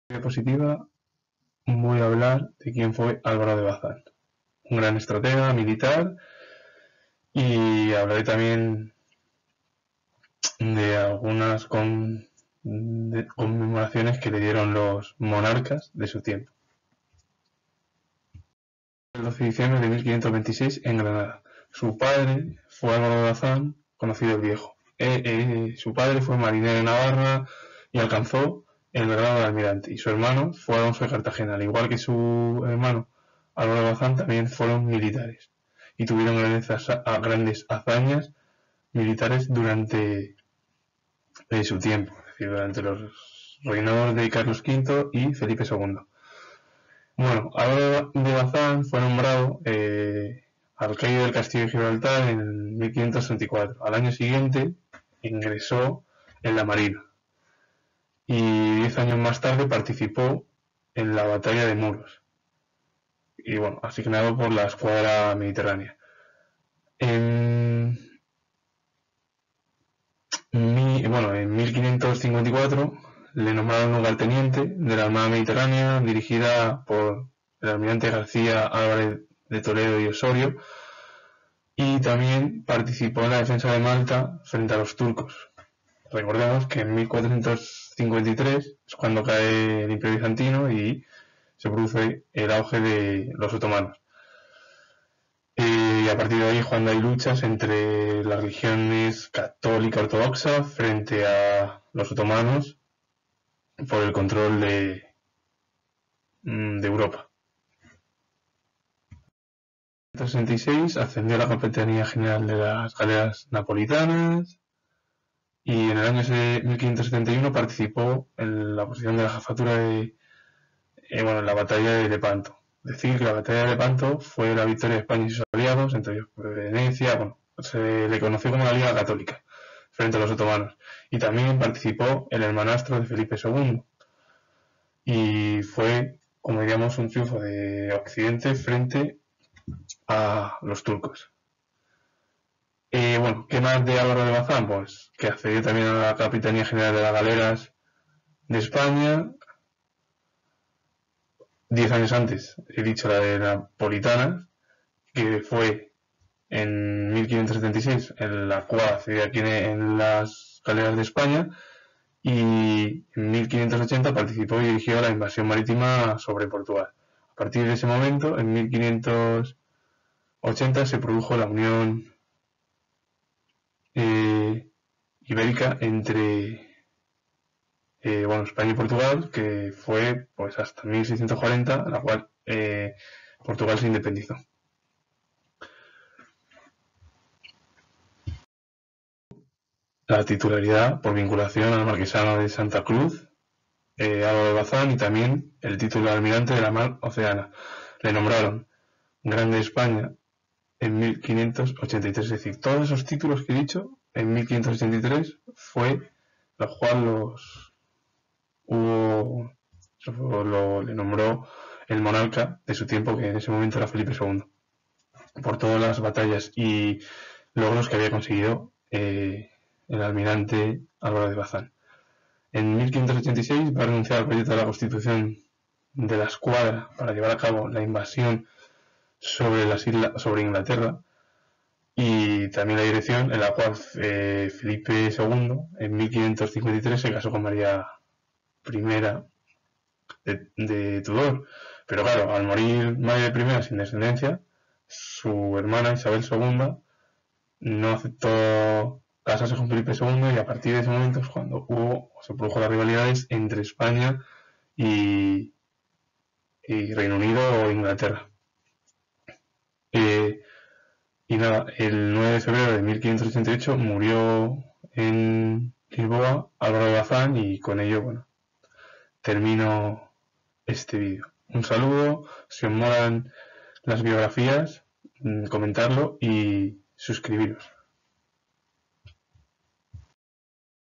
La biografía de Álvaro de Bazán (1526-1588)
La vida de Álvaro de Bazán, las condecoraciones por parte de los monarcas de su tiempo Carlos V y Felipe II. Las hazañas que llevó a cabo.